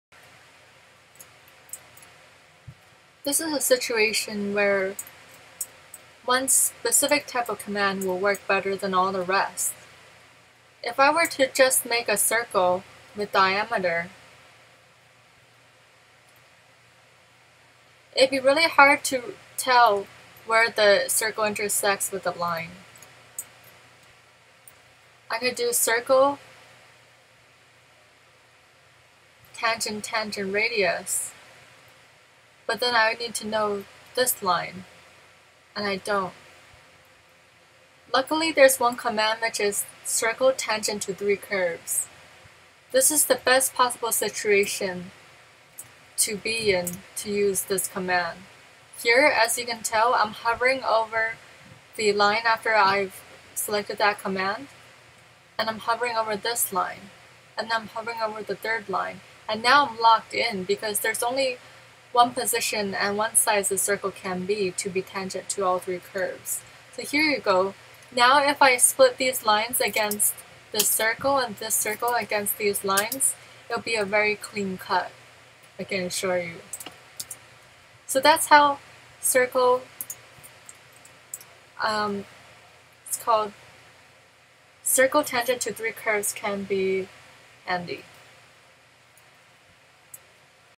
When to Use and How to Use Circle Tangent 3 Curves in Rhino
When you are looking to create a rounded off end for a grate and want a clean cut, it is optimal to use Circle Tangent 3 Curves in Rhino.

Under the Circle toolbar, click on Circle Tangent 3 Curves, then hover and click on the 3 curves you want the circle to flank.

mold3Dacademy.com